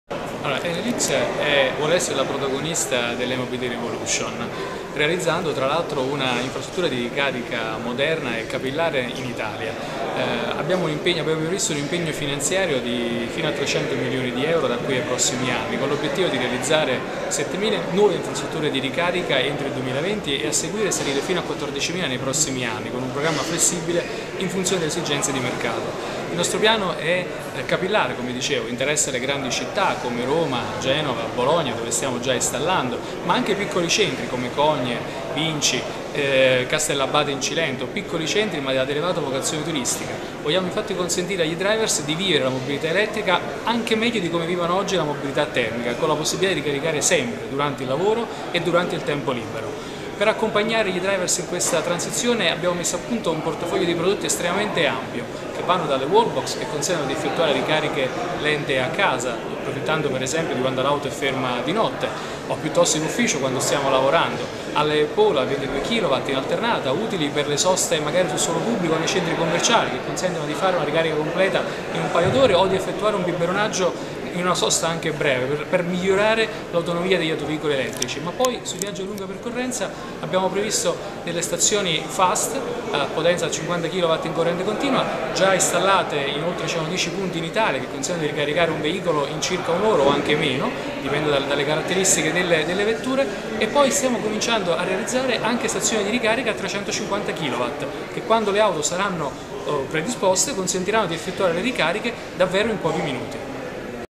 La mobilità elettrica secondo Enel X | PROTECTAweb
In occasione del dibattito avvenuto al Tempio di Adriano per la X Conferenza Nazionale sull'Efficienza Energetica di Amici della Terra,  Luigi Ottaiano di Enel X sottolinea il ruolo dell'elettrico per la mobilità green